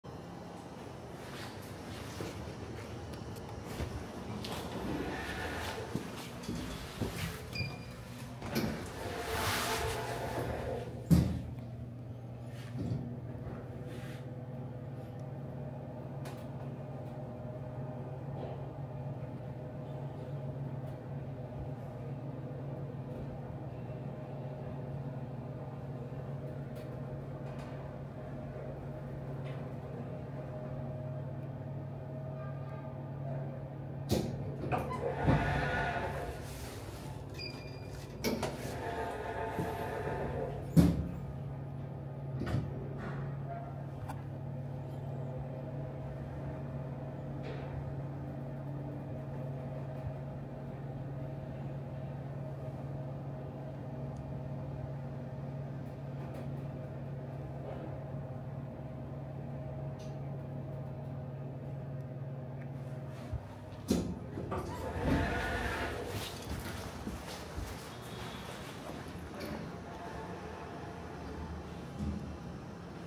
[4K 엘리베이터 탑사기] 광주광역시 서구 치평동 수향메트로빌딩 OTIS엘리베이터 Si1
광주 상무지구 정중앙에 위치한 중형 상가건물로, 롯데마트 맥스 상무점 근처에 있습니다.

▷승강기 소개
엘리베이터는 OTIS엘리베이터의 기어드 모델인 2007년식 Si1 한대가 설치됐습니다. Si1은 당시 OTIS의 주력 기종인 Di1의 고급화 기종으로 내세웠던 모델이었습니다.

빌딩의 규모 치고 꽤 작은 용량으로 설치됐는데, 80~90년대 구축아파트 엘리베이터에서나 볼 법한 8인승/550kg이 적용됐습니다.

내장재는 스테인리스 미러 재질이 적용됐으며, 안내방송/도착 차임은 나오지 않았습니다. 위치표시기는 세그먼트 LED 타입이 적용됐는데, 내부 위치표시기는 COP(운전반)과 결합된 부품이 적용됐어요.

승강기 고유번호: 7029-086
설치시기: 2007년 8월
교체공사여부: X
용도: 승객용
모델: Si1
형식: 권상식 - VVVF
위치표시기 TYPE: 세그먼트 LED
정원: 8인승
적재하중: 550kg
운행구간: 1 ~ 5층
정격 속력: 60m/min